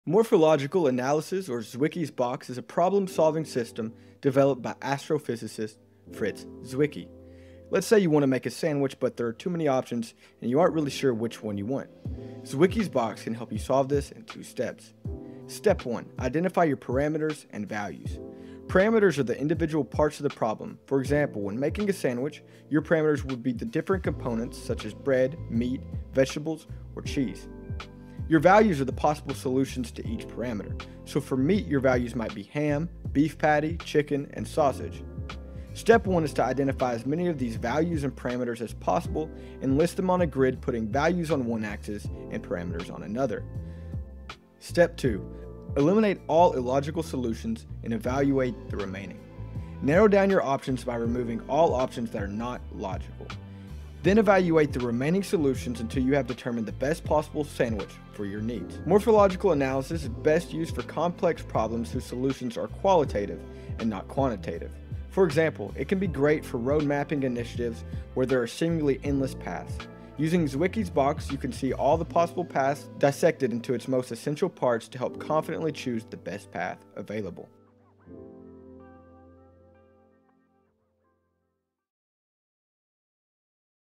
Zwicky Box Explained
A simplified explanation of the Zwicky. The Zwicky box is a simple and effective way to systematically create many unique ideas by breaking the ideas down into categories, adding values to each category, and combining these values to create unique answers.